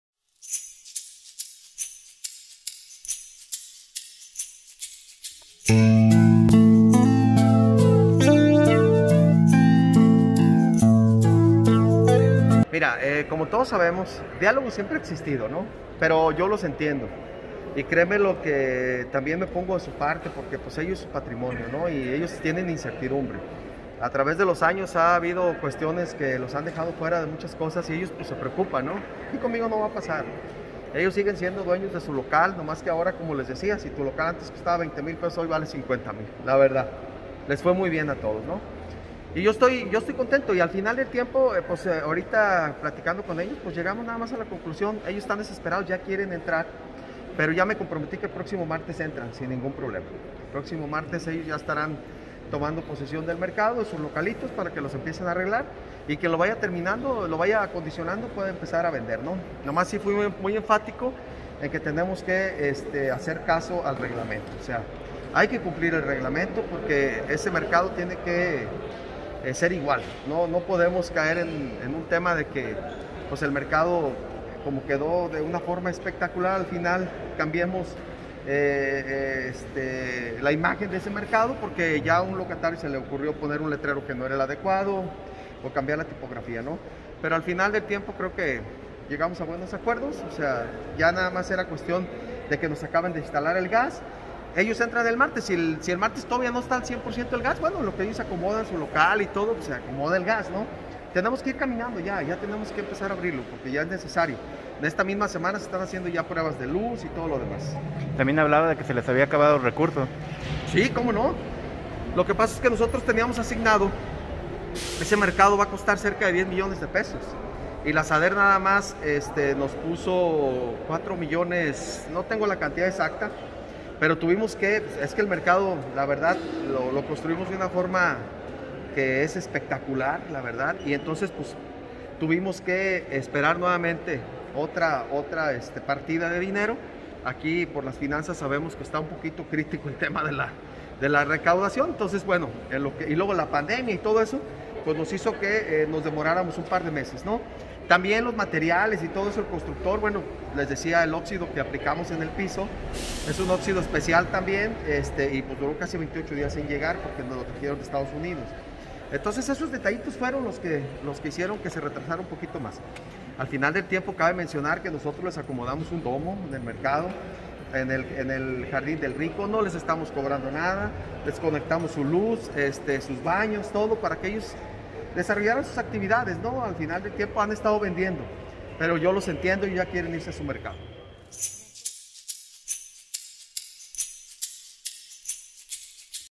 Locatarios volverán el martes: Pepe
Locatarios del “mercadito” se manifestaron la mañana de ayer en el centro, porque se dicen “engañados” y que no les han cumplido con los tiempos para que se arregle su centro de trabajo, y aunque han estado trabajando en el jardín del Rico, ya quieren volver.

La encargada de los mercados, María Magdalena Santiago, comentó que estaban preocupados por no poder volver a trabajar al Mercado Constitución “se les había dado una fecha, pero por motivos del constructor no se les ha entregado el mercado, están en los últimos detalles, como luz, gas, y esos detalles son los que tardan más”, reconoce que la preocupación es que la única preocupación es el que regresen lo más pronto posible.

Por su parte, los locatarios comentaron “no nos han cumplido con las fechas, por eso nos manifestamos pacíficamente, para que nos dé una solución y una fecha”, les habían dado fechas de regresar de mayo, junio y la última el 5 de octubre, y hasta el momento aún no han terminado, los motivos es que faltan “detalles en la obra”.

Comentan que no quieren los contratos, sino seguir con la concesión de derecho que tienen desde hace tiempo, “pedimos que respete nuestra antigüedad”. Comentan que estar en el jardín del Rico, les ha afectado sus ventas, “no ha sido lo mismo, tenemos en riesgo nuestro negocio, nuestro ingreso familiar”.

Sobre la renovación, dice que sí la han visto “no está como nos lo prometieron, no cumple con nuestras necesidades, nos han venido diciendo que será un mercado gourmet, debería ser de acero inoxidable, y no pretiles de cemento, nos sentimos totalmente engañados”.

Otra denuncia que hicieron “en la parte de arriba, tomaron la decisión de incluir gente de su confianza del ayuntamiento, hay familiares de algún regidor, le dieron espacios, gente de confianza del ayuntamiento, no es posible, se pidió que se presentaran solicitudes, para hacer una competencia, no fue así”, dijo el denunciante.

El presidente municipal, Pepe Guerrero, se entrevistó con ellos en el jardín municipal, explicó que el mercado se hizo en dos etapas, “no nos ajustaron los recursos y tuvimos que pedir más, eso nos entretuvo dos meses más, cuando iniciamos la remodelación no sabíamos de este desastre de salud”.

Les recordó que les pusieron un domo, baños, y luz, “les hemos atendido”, dijo el alcalde. Explicó que esta semana se sella el piso “quiero que entremos a un mercado que ya este listo, falta instalar el gas, esta semana se termina de instalar, se están haciendo las pruebas eléctricas, ya está la esta semana se hacen pruebas de instalación de gas, el próximo martes entran al mercado, sin escusa ni pretexto, para que comiencen a acomodarse, es necesario seguir caminando”.

Reiteró que seguirán con su concesión, los del contrato serán los nuevos locatarios porque no son dueños. El alcalde negó que vaya ver incrementos de los impuestos que les cobran.

Le pidieron el compromiso de que el martes puedan regresar, y se lo pidieron por escrito. Se espera que el martes 3 de noviembre a las 10 de la mañana, comiencen a regresar a instalarse.